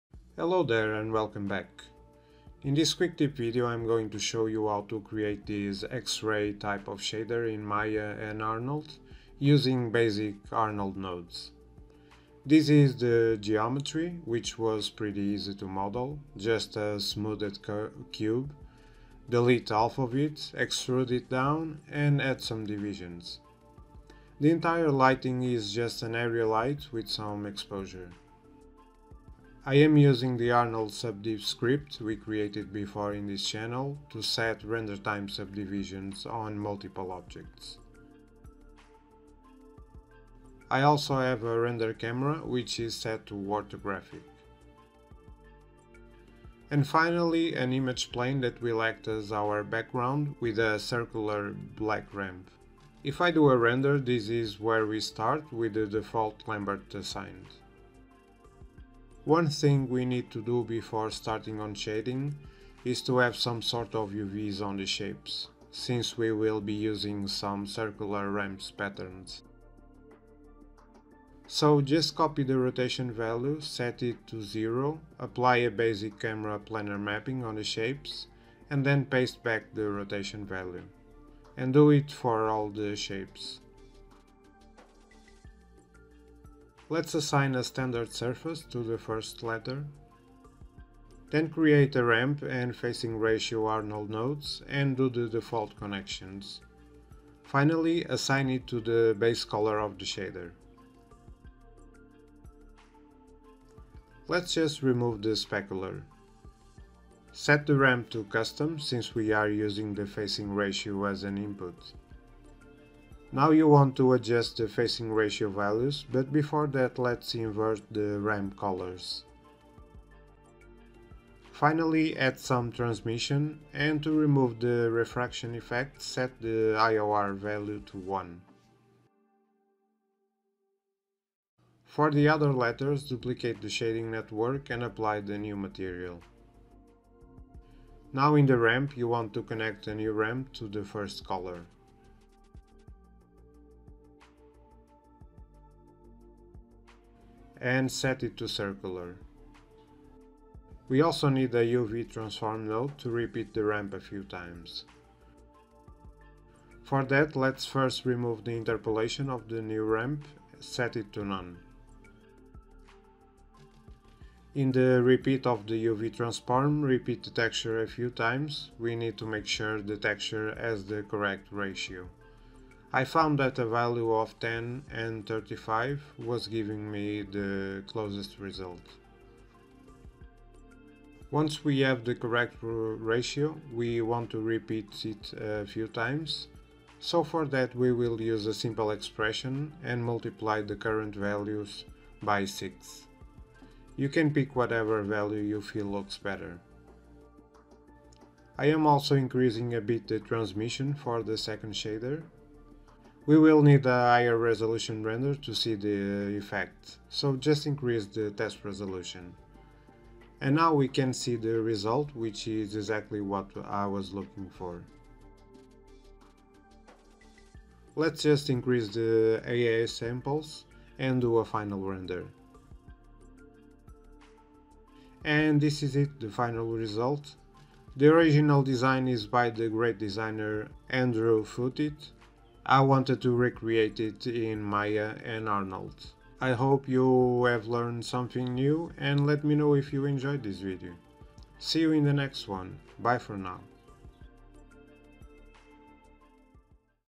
Typography X-ray Shading in Arnold
In this quick tip video I am going to show you how to create an x-ray type of shader in Maya and Arnold, using ramps and facing ratio nodes.

Let me know if you enjoyed this video.

Cheers.